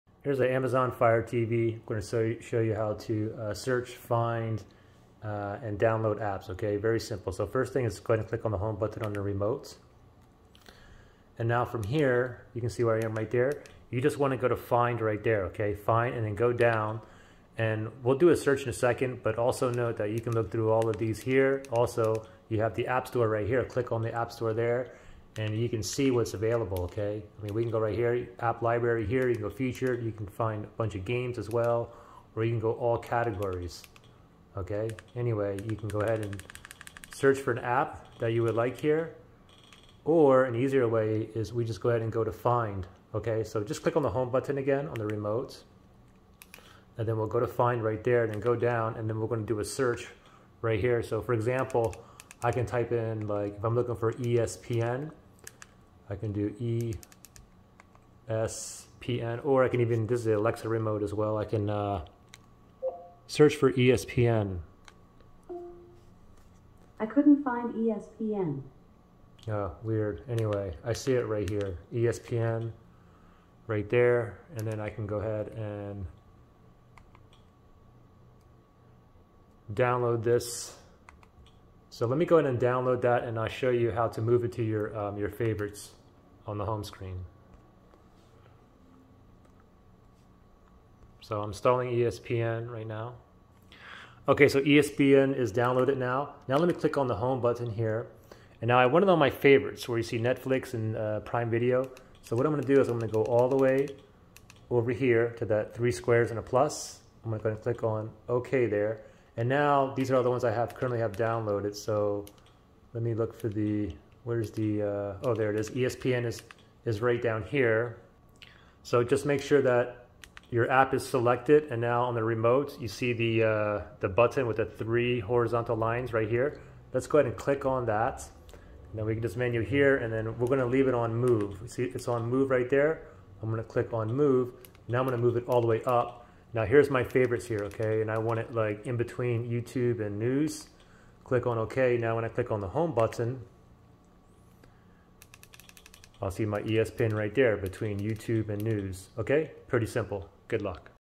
Amazon Fire TV: How to Search, Find, Download Apps
I show you how to search, find, download, install apps on the Amazon Fire TV. Hope this helps.

--- Amazon Fire TV 4-Series 4K UHD smart TV ---